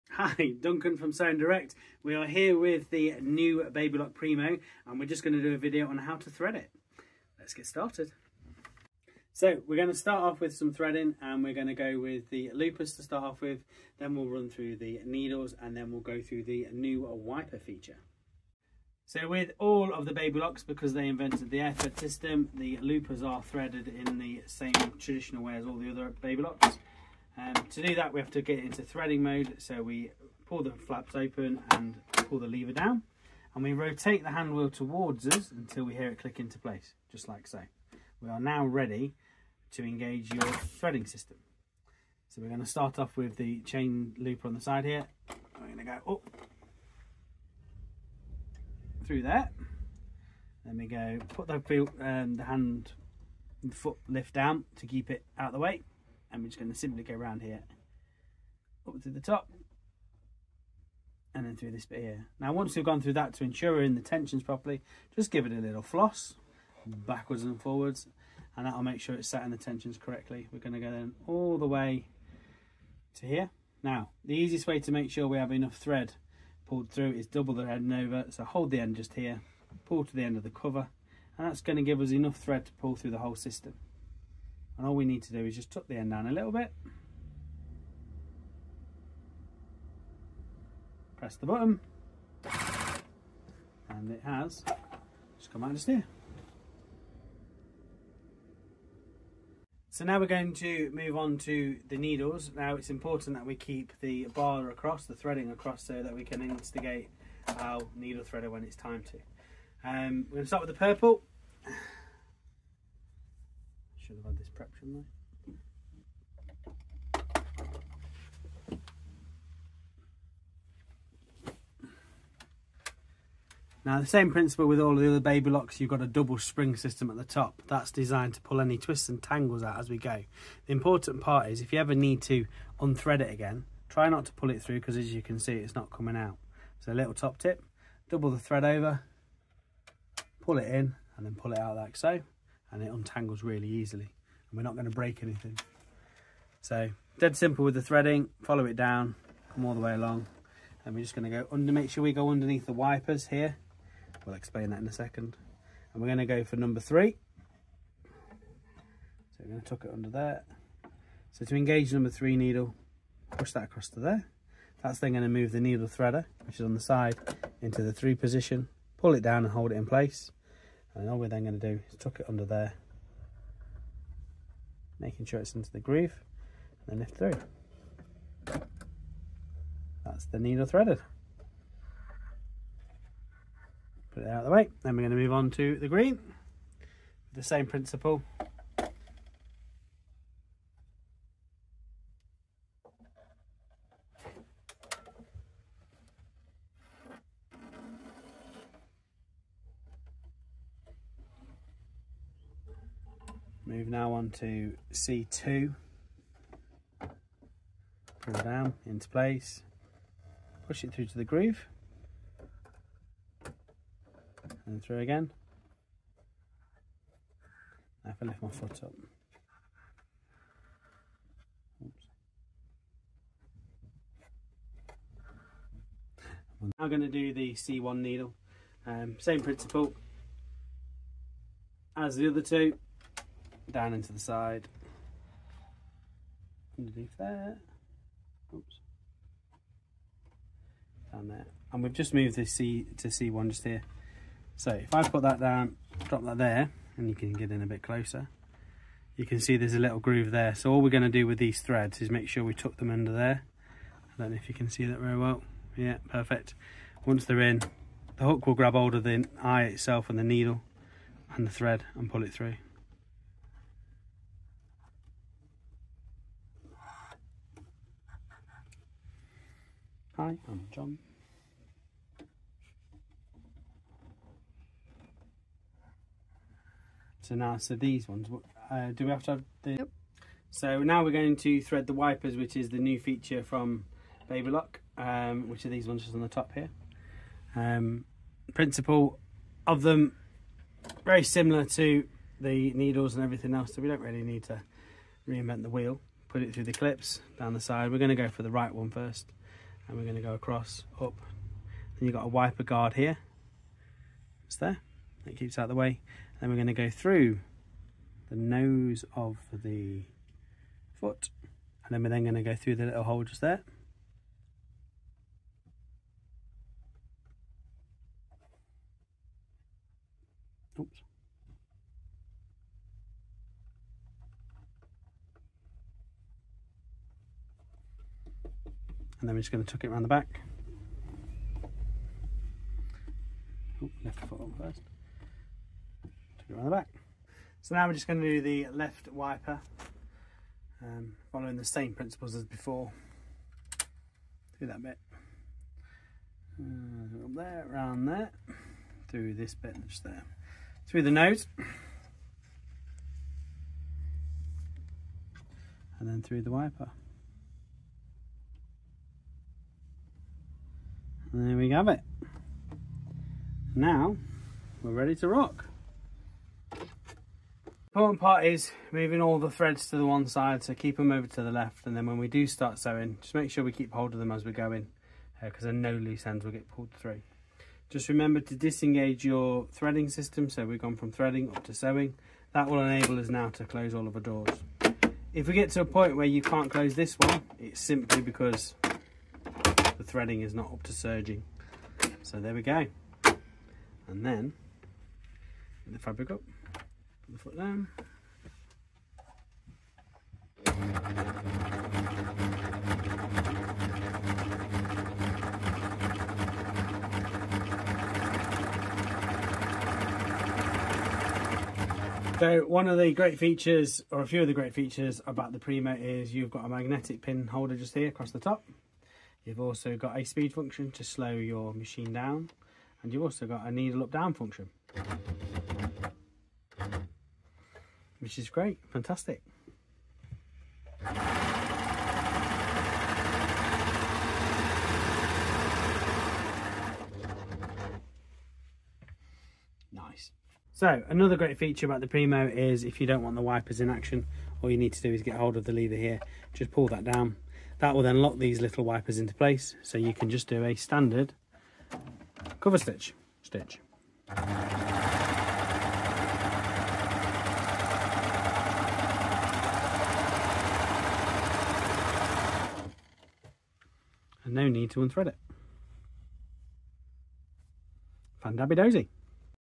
Baby Lock Primo
A video on how to thread the baby lock promo

Exploring the Baby Lock Primo: A Must-Have for Creative Sewers

The Baby Lock Primo is a revolutionary coverstitch machine designed for creative sewing enthusiasts.

Featuring the innovative swing foot, it allows users to combine topstitch threads for stunning curved and serpentine stitches. With options for eight stitch variations, including decorative threads, the Primo brings versatility to every project.

Additionally, future accessories will enable stitching with ribbons, lace, and cords. As always, Baby Lock promises ease of use, high quality, and professional results, making the Primo an exciting choice for any sewer.